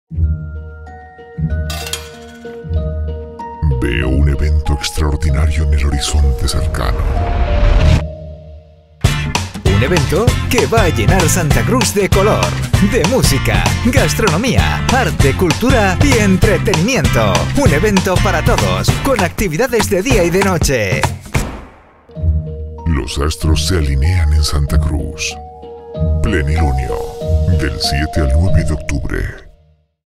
🌝 Spot promocional Plenilunio Santa Cruz 2022 💚💙
🔝 Santa Cruz de Tenerife albergará, del viernes 7 al domingo 9 de octubre, una nueva edición de "Plenilunio Santa Cruz" con más de un centenar de actividades para todos los públicos.